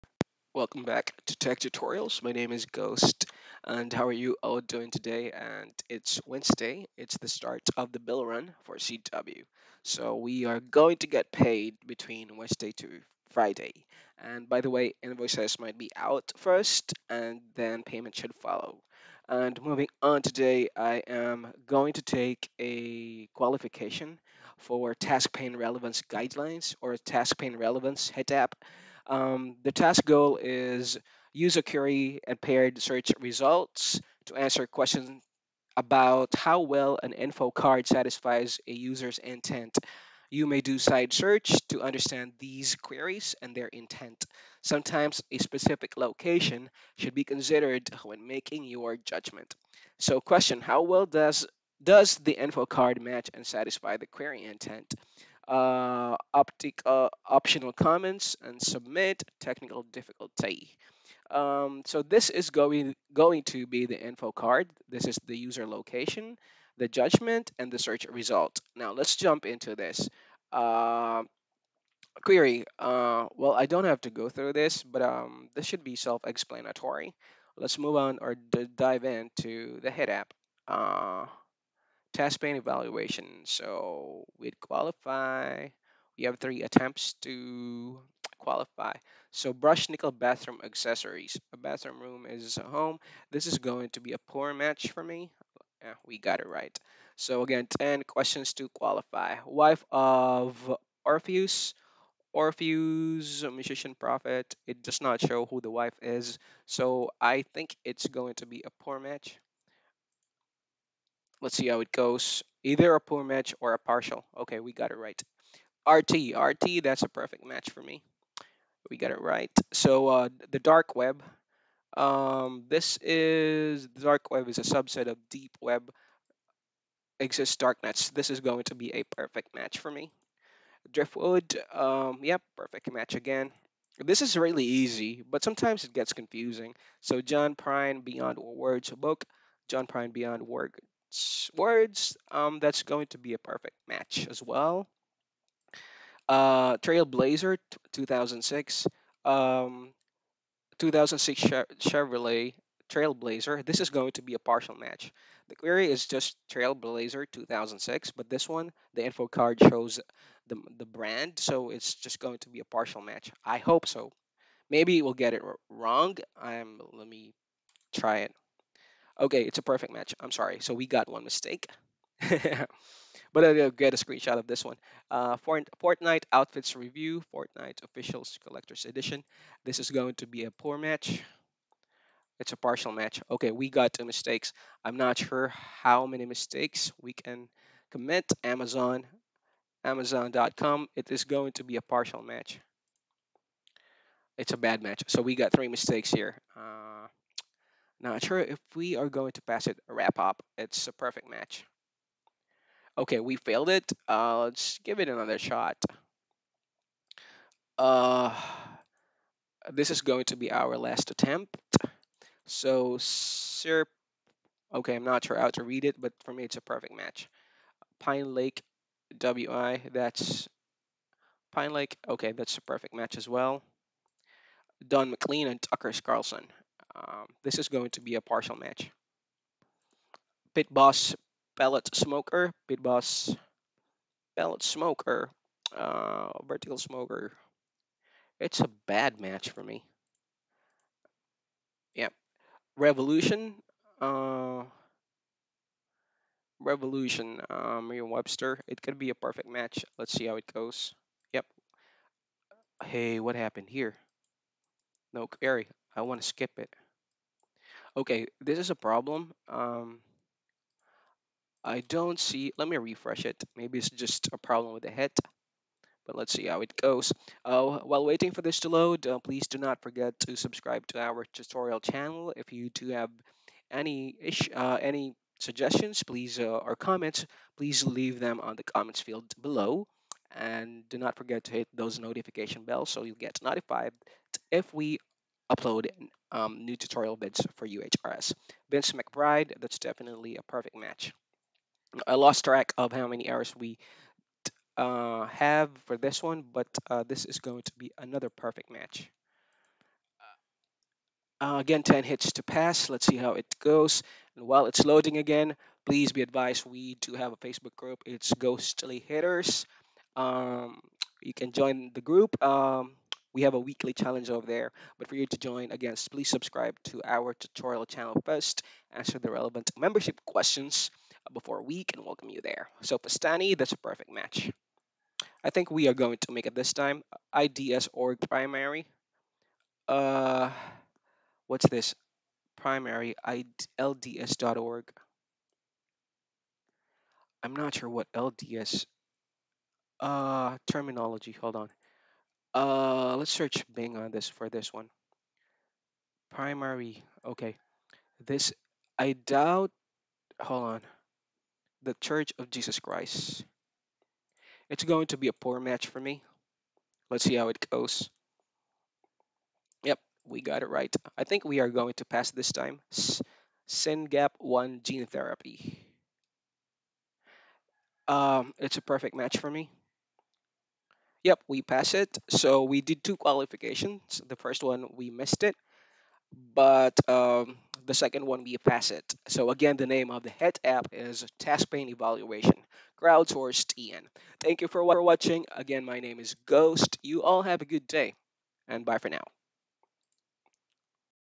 UHRS HITAPP Easy Hitapp Qualification TaskPane Evaluation (Crowd Sourced) - EN
Here is the qualification video on UHRS TaskPane Evaluation (Crowd Sourced) - EN.